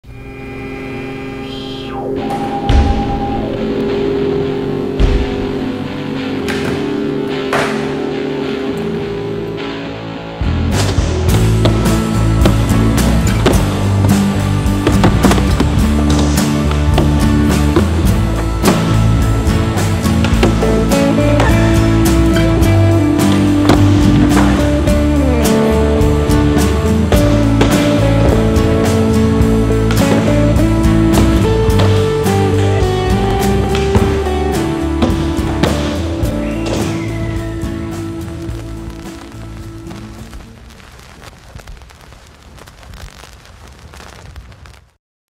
Manny Mondays: Ben Fisher - TransWorld SKATEboarding
Ben Fisher rolled through and took the Vans manny box to a higher level, literally.